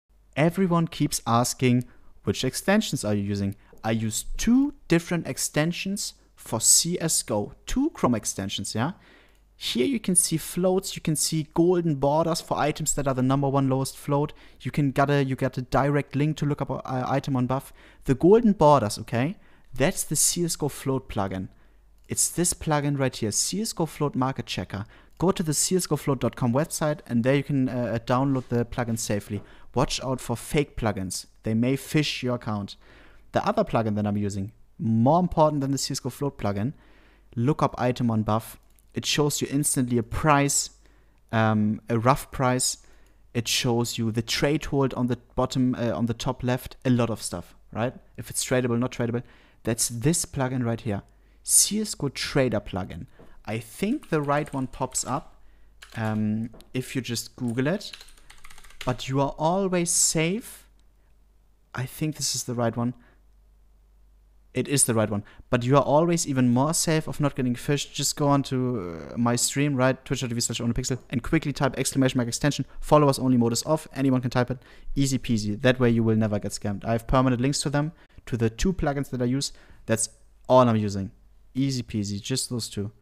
best extensions for csgo skins
i use two different extensions for csgo trading:
CSGOFloat Market Checker & CSGO Trader - Steam Trading Enhancer